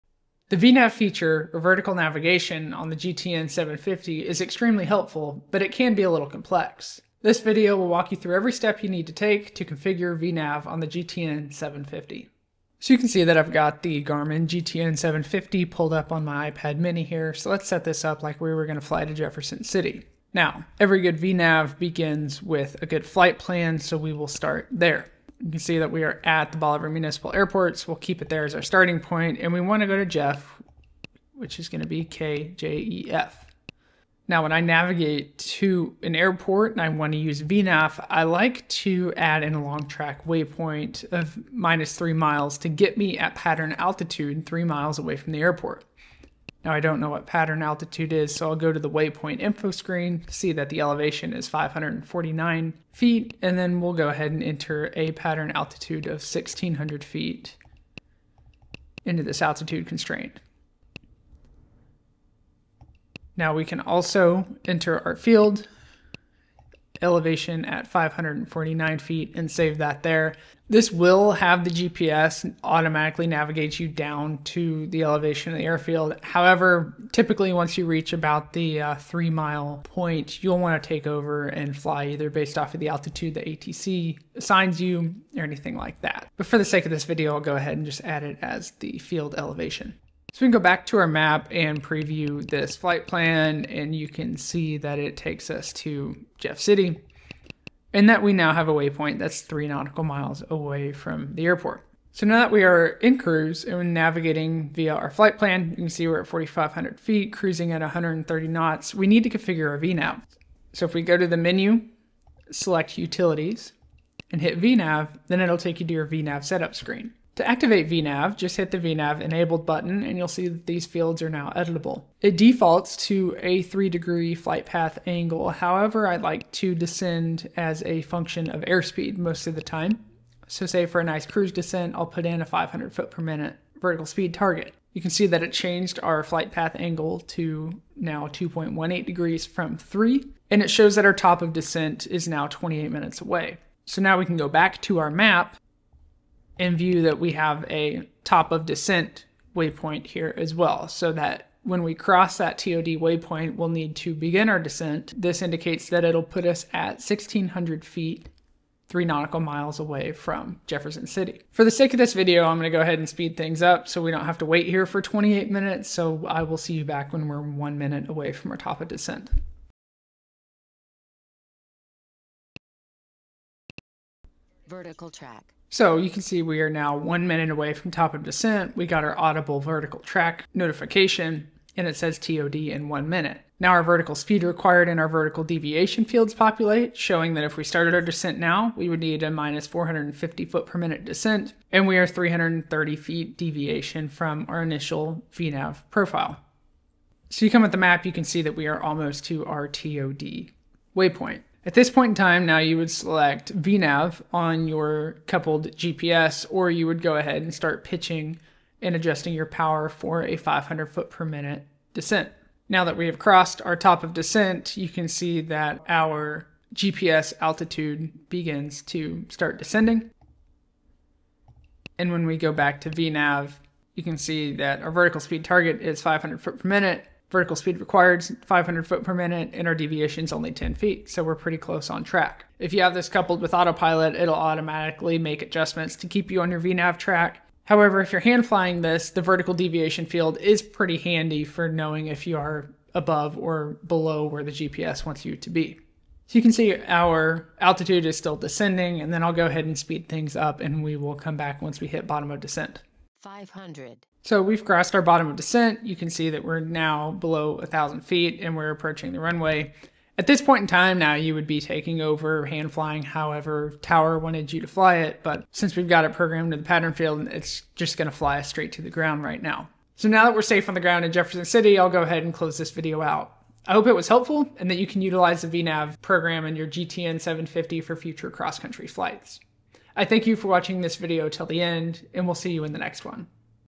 Garmin GTN 750 VNAV Tutorial: Easy Descent Planning for Cross-Country Flights
Learn how to master the VNAV (Vertical Navigation) utility on your Garmin GTN 750! This tutorial will guide you through using VNAV for seamless descent planning during VFR cross-country flights.

Discover how to efficiently calculate your top of descent and accurately plan your vertical profile, saving you time and enhancing your situational awareness. If you have a GFC 500 or GFC 600 autopilot, you'll see how VNAV can automatically execute your descent, making long flights even more comfortable and efficient.

This video covers:

Flight Plan Configuration: Setting up your flight plan for VNAV.
VNAV Parameters: Understanding and adjusting key VNAV settings.
VNAV In Action: Demonstrating a simulated VNAV descent.
Perfect for pilots looking to leverage the advanced features of their GTN 750, this tutorial will help you fly smoother and safer.

Chapters:
0:00 - Introduction
0:20 - Flight Plan Configuration
1:42 - VNAV Parameters
2:52 - VNAV In Action